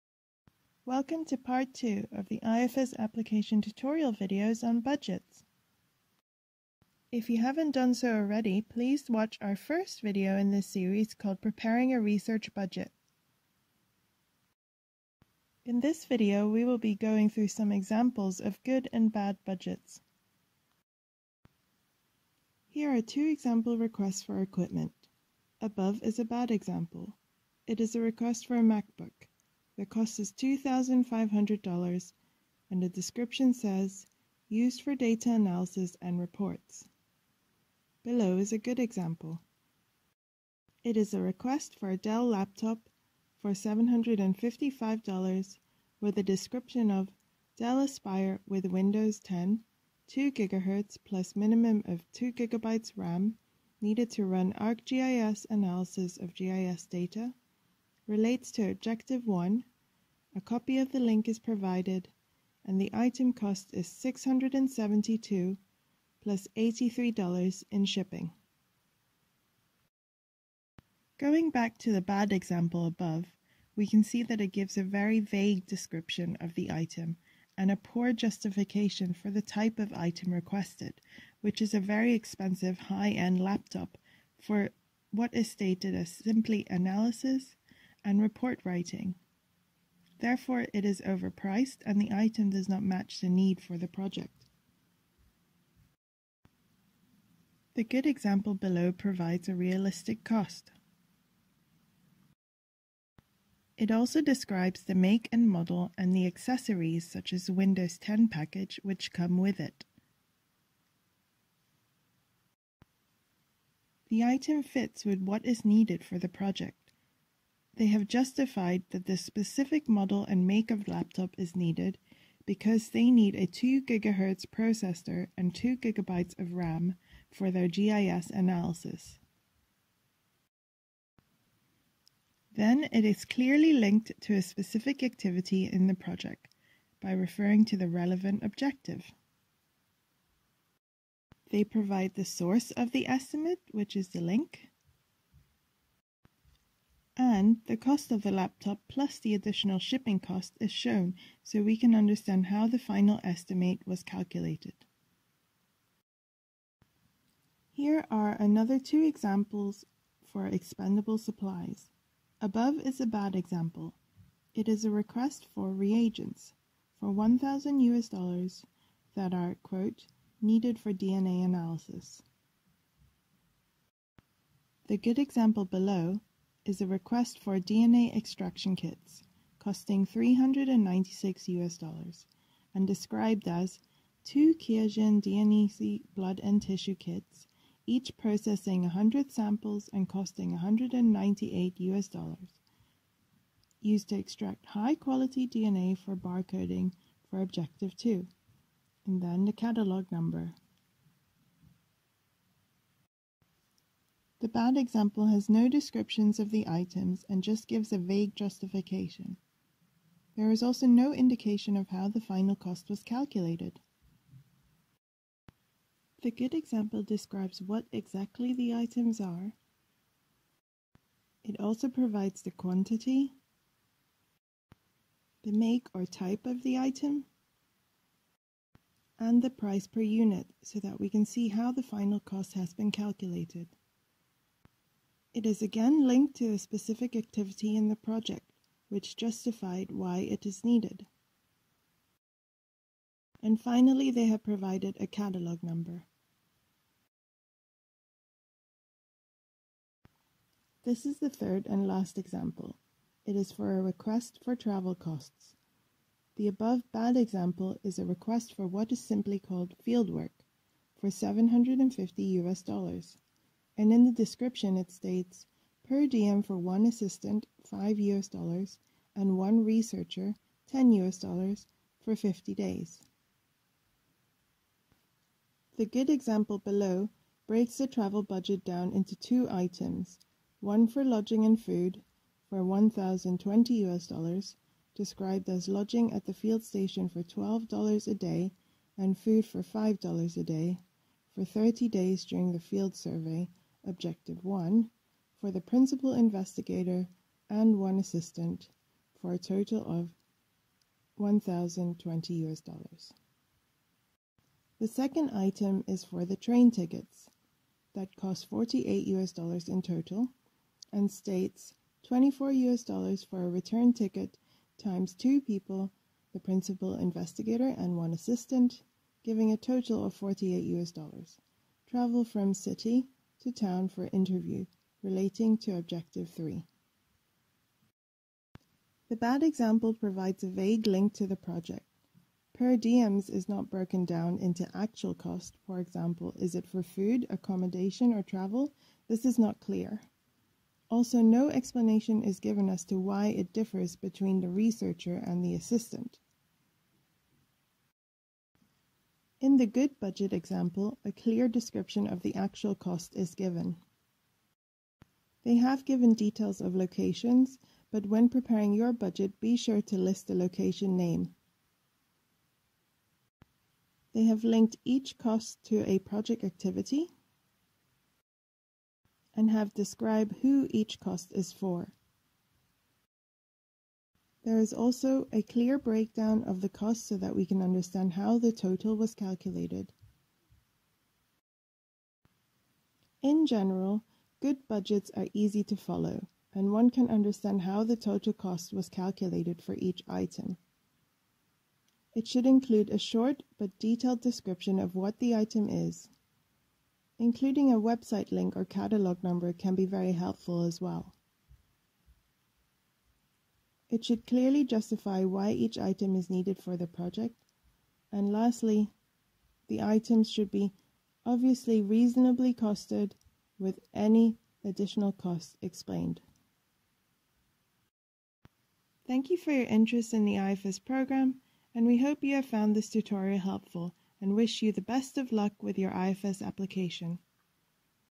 IFS Application Tutorial - 2: Good & Bad Examples of Budgets
This is the second video of a two-part series on preparing your IFS Research Budget by the International Foundation for Science (IFS). Here we go through some good and bad examples of research budgets.

If you are planning to apply for an IFS grant, please watch these video tutorials before submitting your proposal.

For more information on the IFS Programme and our research grants please visit our

The IFS Application Guidelines can be accessed by logging into the IFS community

This video is recorded by Jennifer Sjölund.